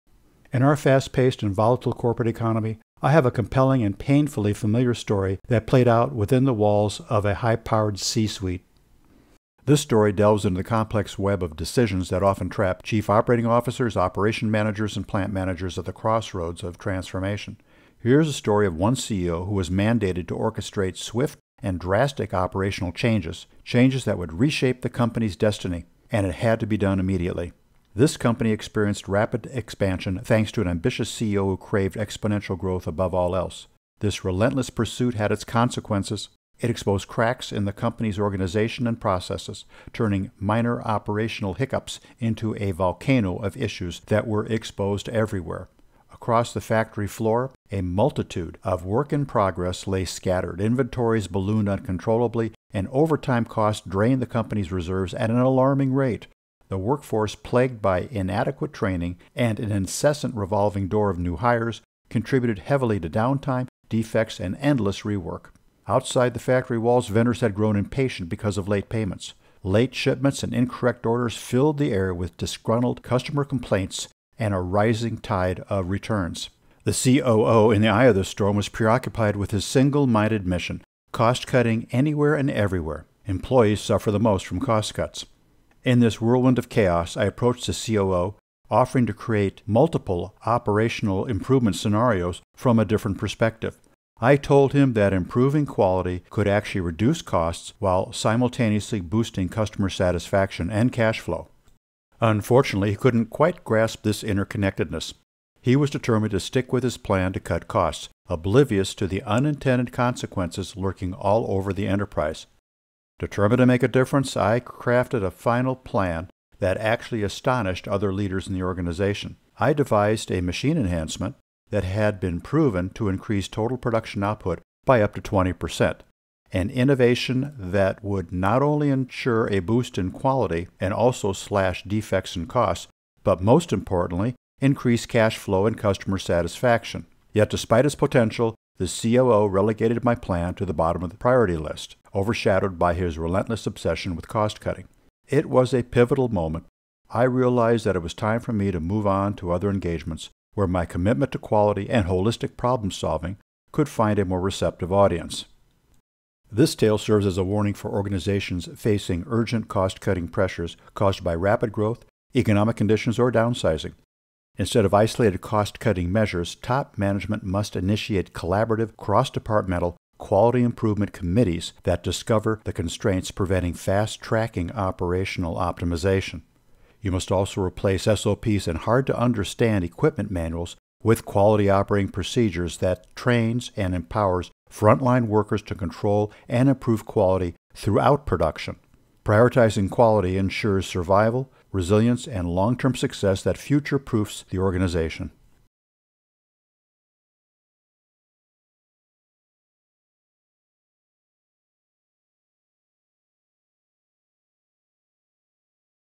Stories from the C-Suite: The COO who chose cost-cutting over quality improvement
Pressure and fear from top management can drive operations managers to choose short cuts that can harm a company and prevents planning for what is really best for stability and long-term growth.

"Choosing cost cuts over quality is like inviting your customers to do business with someone else." G. Mark Alarik

v1cc